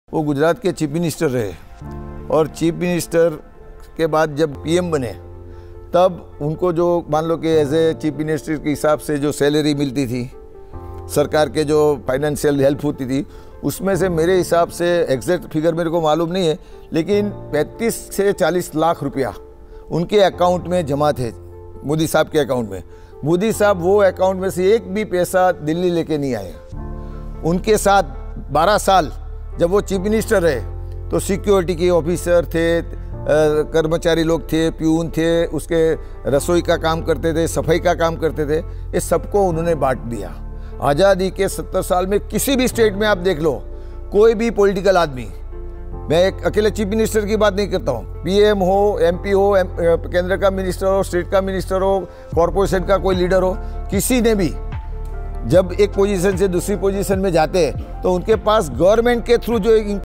PM Modi's parting gift when he was the CM of Gujarat | Modi Story
Rajya Sabha MP and former Deputy Chief Minister of Gujarat, Shri Narhari Amin Ji spoke about Narendra Modi’s unique parting gift to his staff in Gujarat before he became the Prime Minister of India. The entire surplus income which Narendra Modi had received, which was about Rs. 35 to 40 lakhs, he donated it among security officers, peons, cook and janitor who had worked with him for 12 years in Gujarat.